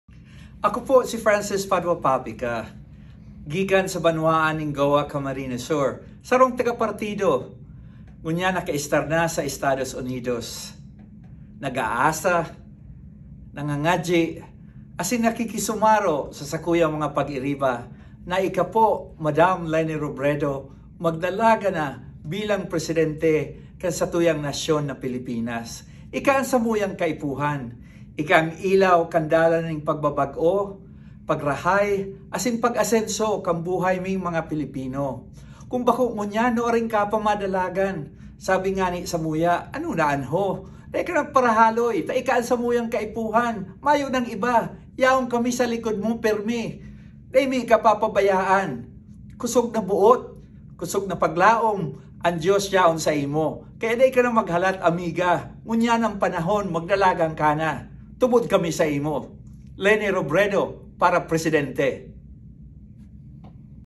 Atty. Francis Papica Expresses Full Support for VP Leni
US-based lawyer and philanthropist Francis Papica expresses full support for VP Leni and encourages her to run for President in the 2022 Philippine elections. Francis Papica is a good friend of the Vice President and has a strong following especially in Goa and other parts of Partido Area in Camarines Sur, Bicol Region, among other areas.